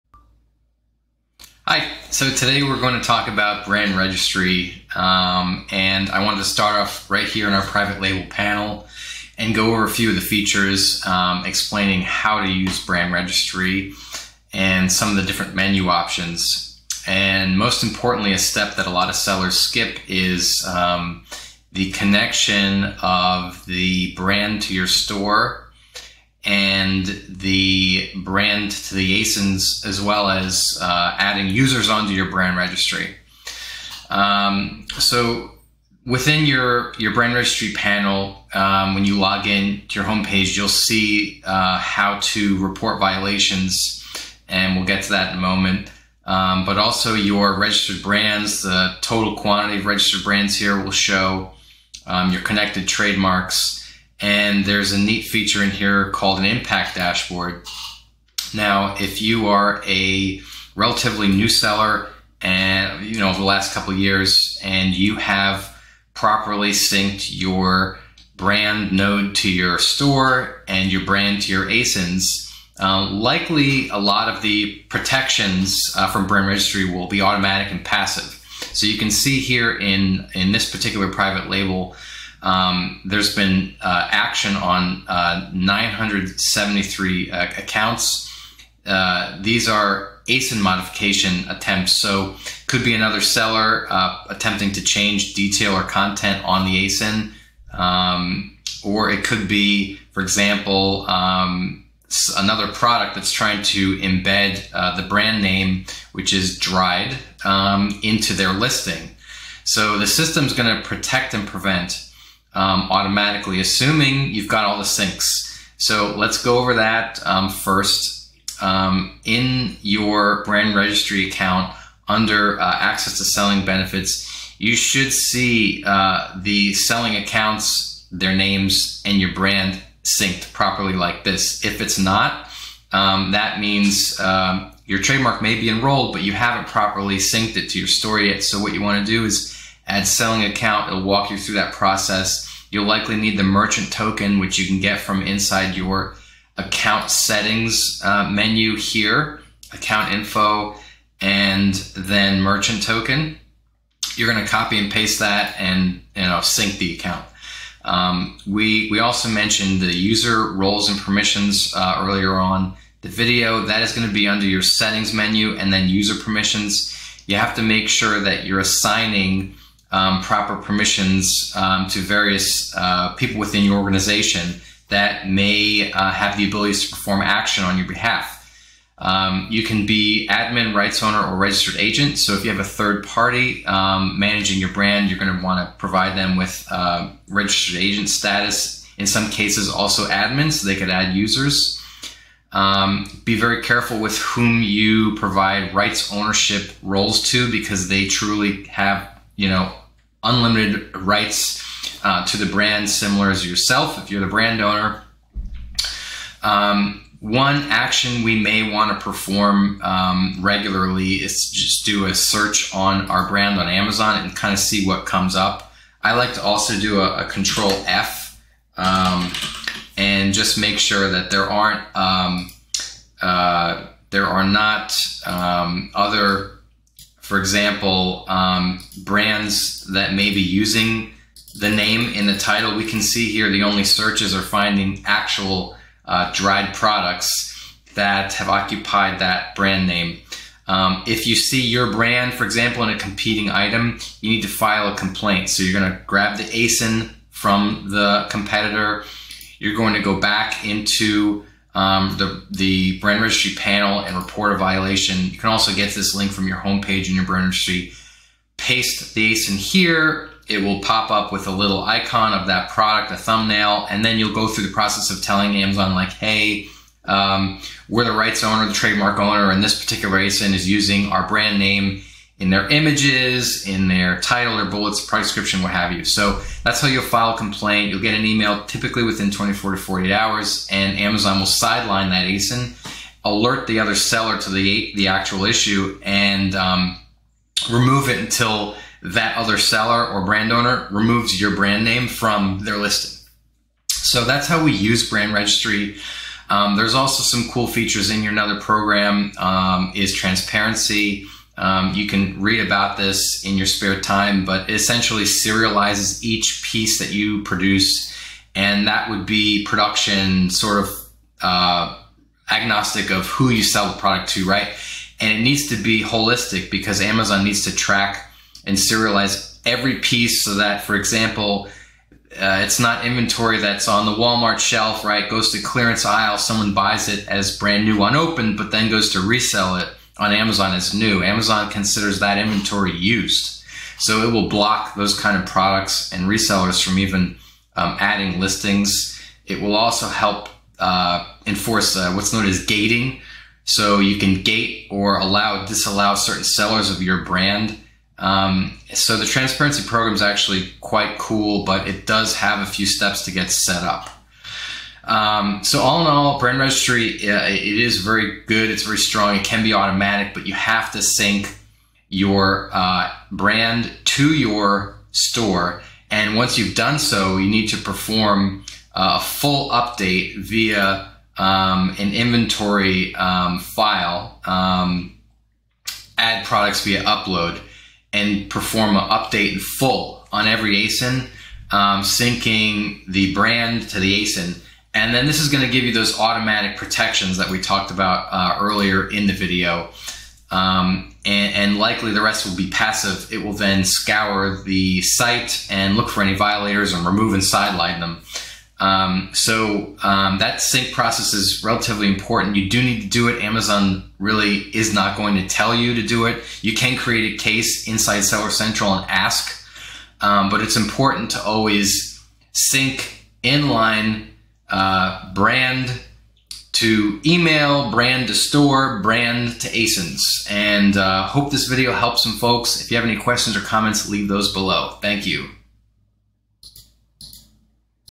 Amazon Brand Registry - How to Sync Your Brand To Your Store and Your Brand To Your Amazon ASIN's
Welcome to our comprehensive guide on Amazon Brand Registry! In this video, we will walk you through the essential steps to harness the full potential of this program. We'll cover everything from checking your impact dashboard, syncing your brand to your store, to synchronizing your brand with existing ASINs using a full update inventory file in Seller Central. Additionally, we will delve into the Amazon Transparency Program and explain why it is a vital tool in combating counterfeit sellers.

🔍 Part 1: Checking Your Impact Dashboard
First things first, we will show you how to access and navigate your impact dashboard within the Amazon Brand Registry. This dashboard provides invaluable insights into the performance and impact of your brand on the marketplace. Learn how to analyze crucial data such as customer reviews, search terms, and geographic distribution to optimize your brand strategy and drive sales.

🛒 Part 2: Syncing Your Brand to Your Store
Discover the step-by-step process to seamlessly sync your brand with your Amazon store. By aligning your brand and store, you gain greater control over your product listings, enhanced brand visibility, and the ability to utilize powerful marketing features. We will demonstrate how to set up your brand store, customize your page layout, and showcase your brand story effectively.

🔗 Part 3: Syncing Your Brand to Existing ASINs with a Full Update Inventory File
If you already have existing ASINs that need to be associated with your brand, fear not! We will guide you through the process of syncing your brand with these ASINs using a full update inventory file in Seller Central. Learn the best practices to streamline the synchronization process, ensuring consistent brand representation across your entire product catalog.

🛡️ Part 4: Understanding the Amazon Transparency Program
Counterfeit sellers pose a significant threat to brands and customers alike. That's where the Amazon Transparency Program comes into play. Discover how this innovative program empowers brands to protect their intellectual property and build trust with customers. We will explain how to enroll in the program, generate Transparency codes, and enforce product authentication, ultimately safeguarding your brand's reputation.

By the end of this video, you will have gained a comprehensive understanding of Amazon Brand Registry and the essential tools it offers. Equip yourself with the knowledge to navigate the impact dashboard, sync your brand to your store, and safeguard your brand's authenticity through the Amazon Transparency Program. Join us on this journey to protect your brand, enhance your customer experience, and unlock new growth opportunities on Amazon's expansive marketplace. Don't miss out on the chance to take control of your brand's destiny. Watch now!